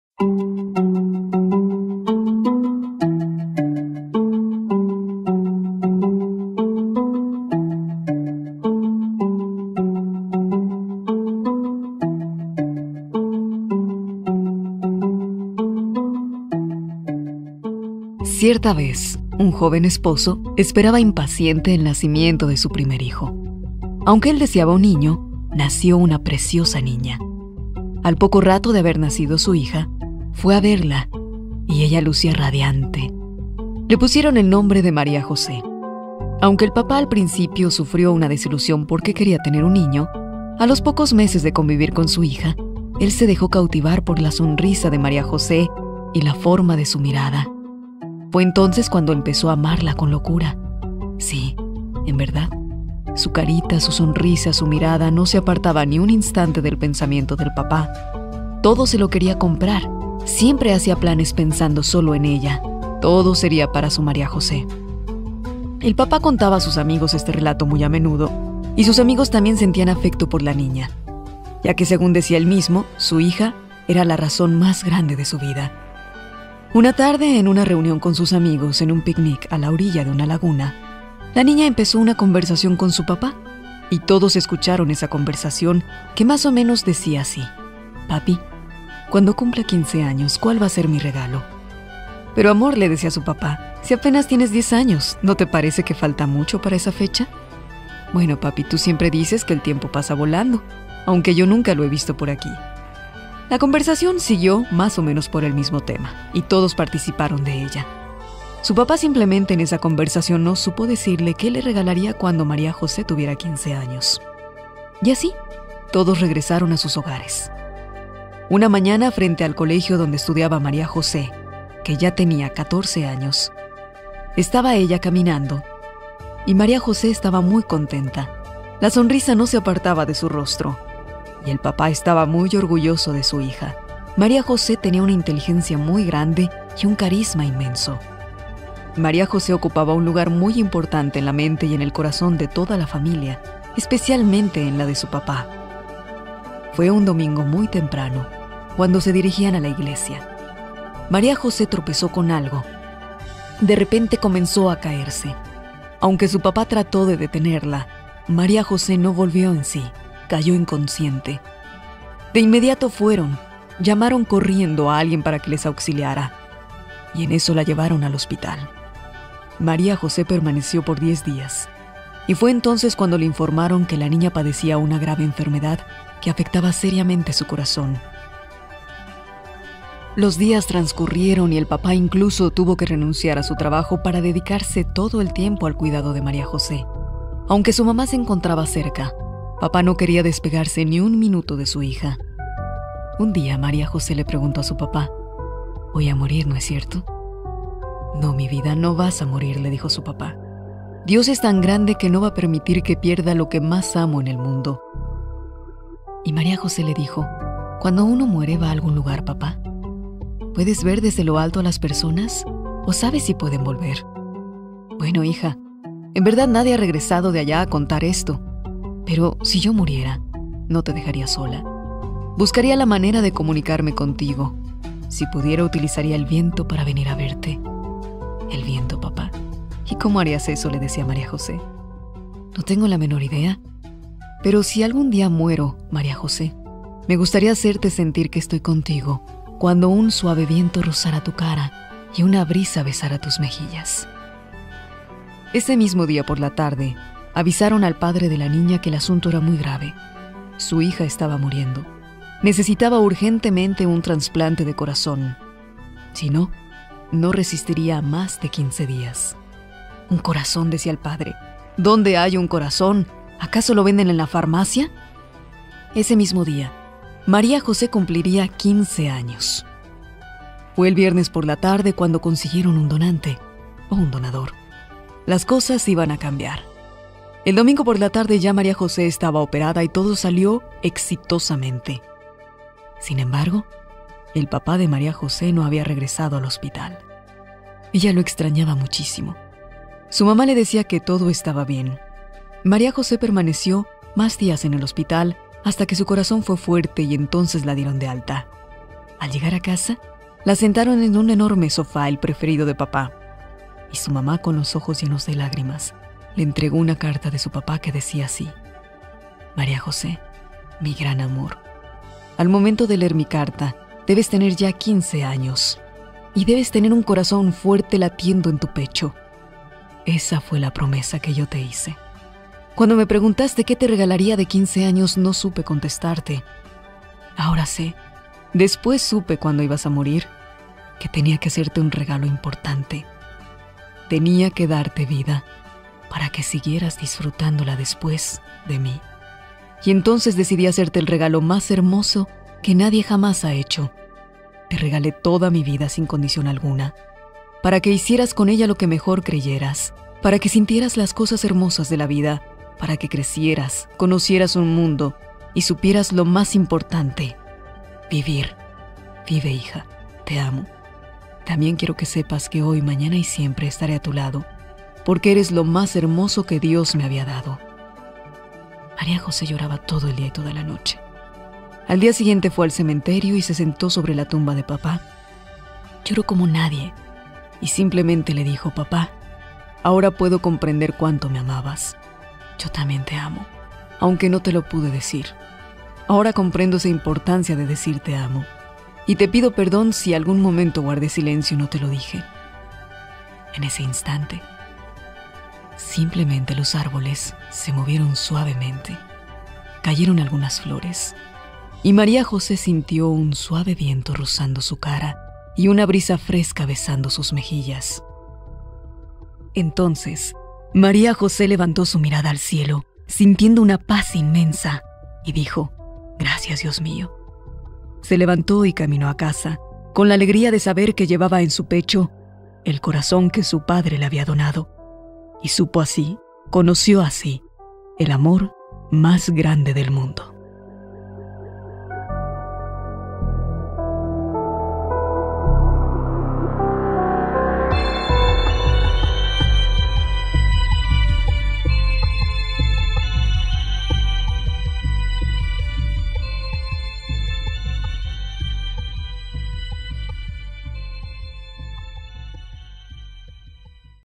ROCÍO BRAUER - EL AMOR MÁS GRANDE DEL MUNDO
Para ti, cual sería el amor más grande del mundo y de qué manera se lo demostrarías.  Una hermosa reflexión que hago tuya